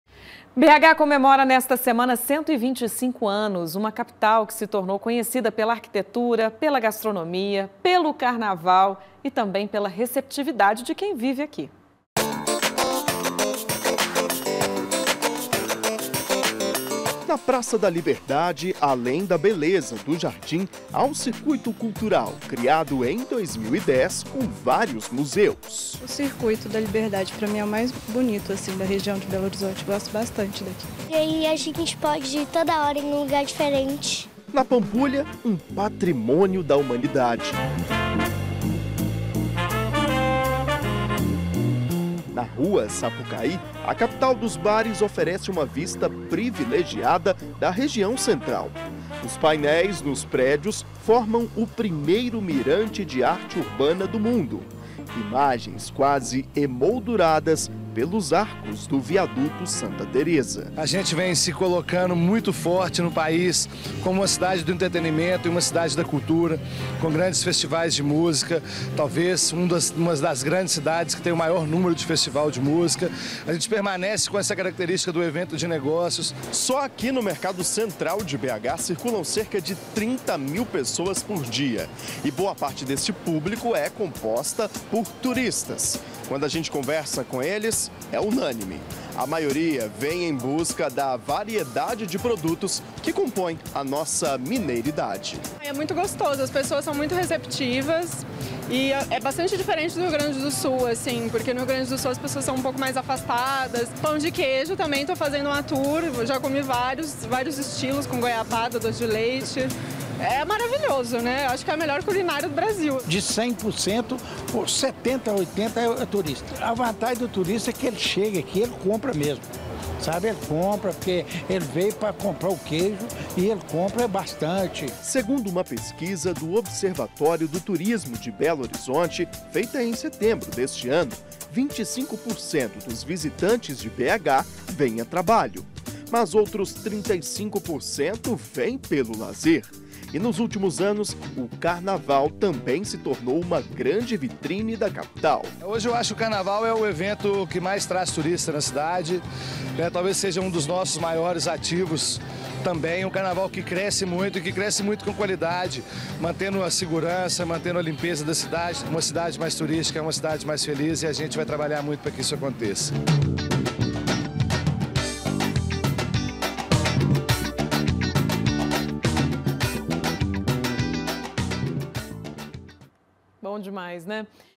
BH 125 ANOS: CAPITAL SE TORNOU PONTO TURÍSTICO
Ao longo de seus 125 anos, Belo Horizonte se tornou uma clássica rota turística. A reportagem dessa quarta-feira (14) destacou os locais imperdíveis. Confira.